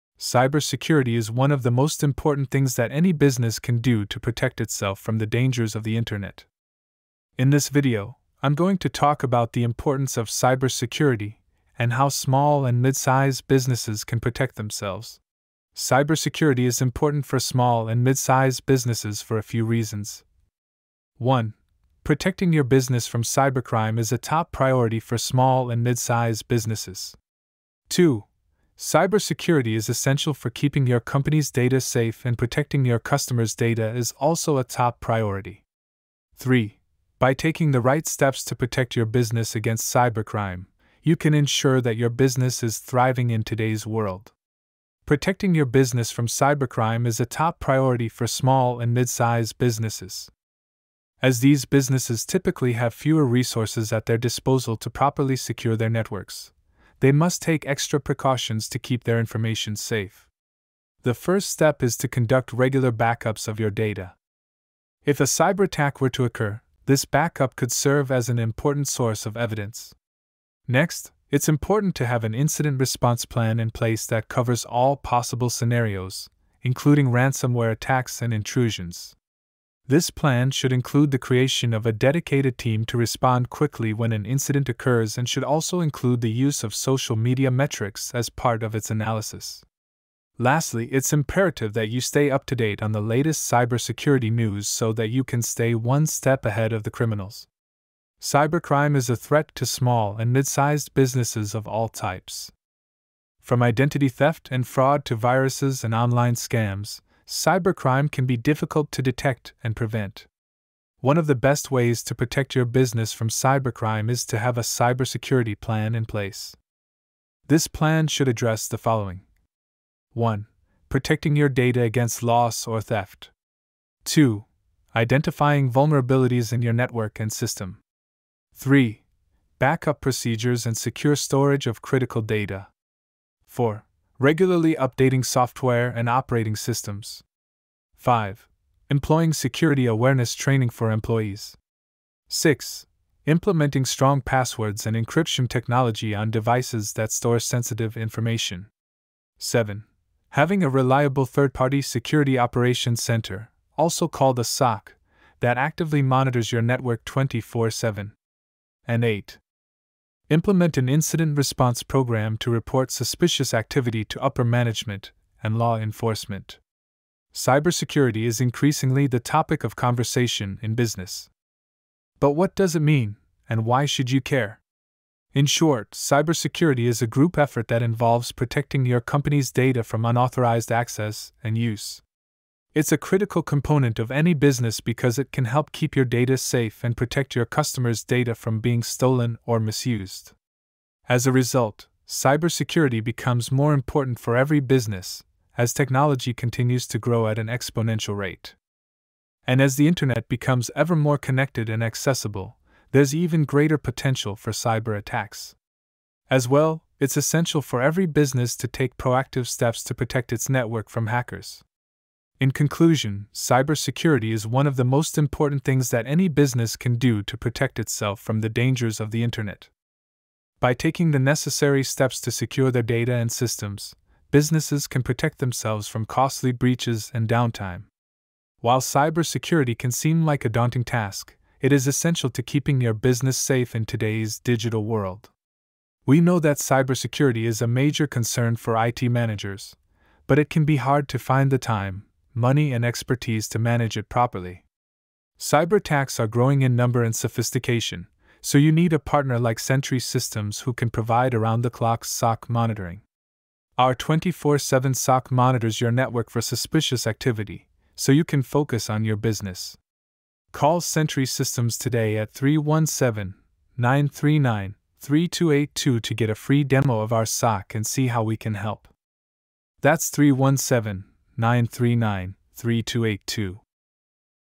The Many Benefits of Cyber Security for Small Businesses
There are many benefits of cyber security for small businesses. This video will show you how to protect your business from online threats. Watch the video to learn more about cyber security and how it can help your business.